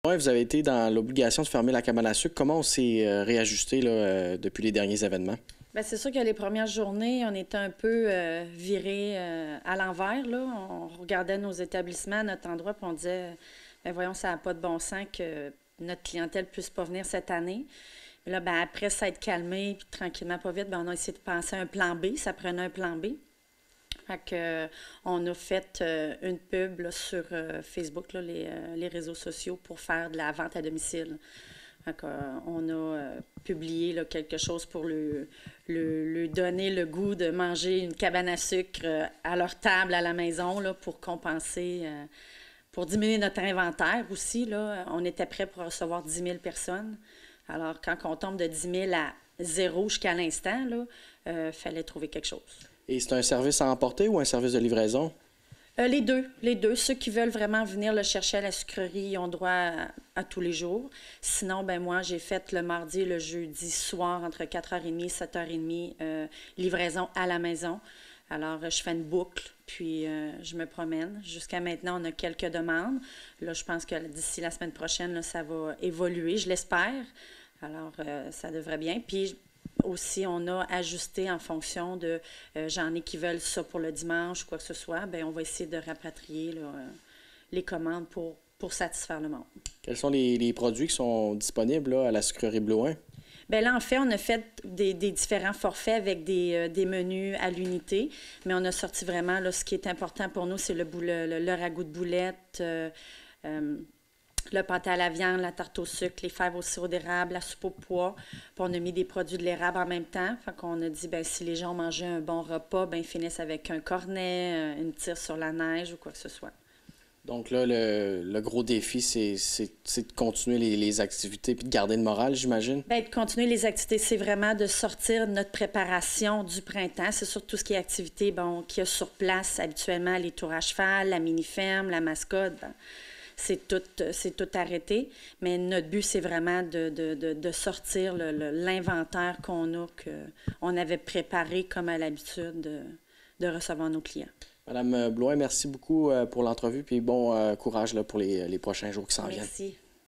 Cabane à sucre à la maison
Depuis la nouvelle directive gouvernementale, les cabanes à sucre ont été dans l'obligation de fermer leurs portes. Plusieurs cabanes à sucre de la région ont décidé de proposer un service vente à domicile. C'est le cas de la Sucrerie Blouin de Saint-Jean-de-l'Île-d'Orléans. Entrevue avec la propriétaire Julie Blouin.